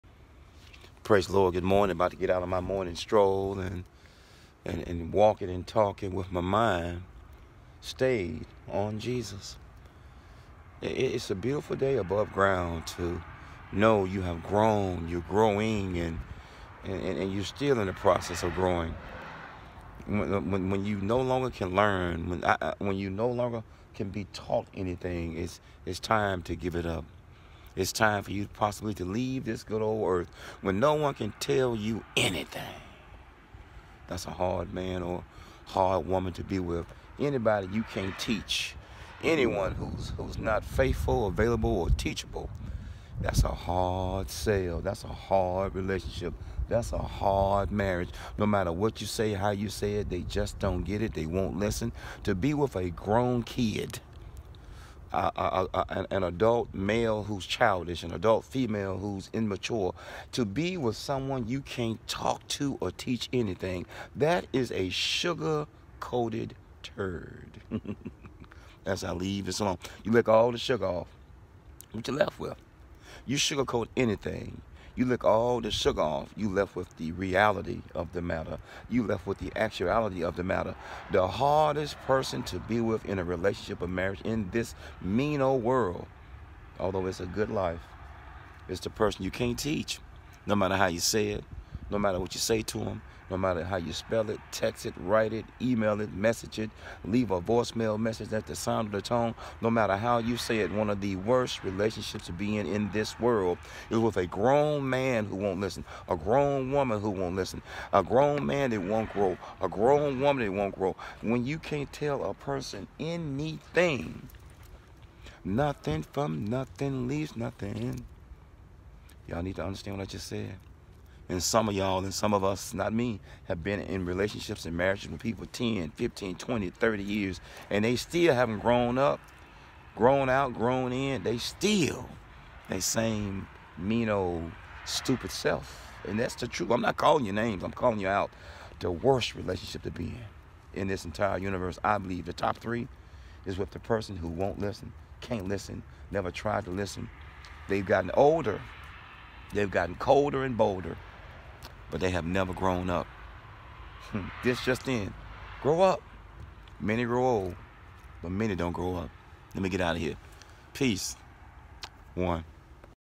“To Be With Someone You Can’t Talk To!!!” Terry’ Davis
“To be with someone you cannot talk to or teach is an awful relationship and/or marriage to be in to the END! It’s a hard sale to try to convince someone who just won’t listen! Many have grown old! But unfortunately at the same time many have not grown up! We have to be faithful, available, and teachable!!!” T. Davis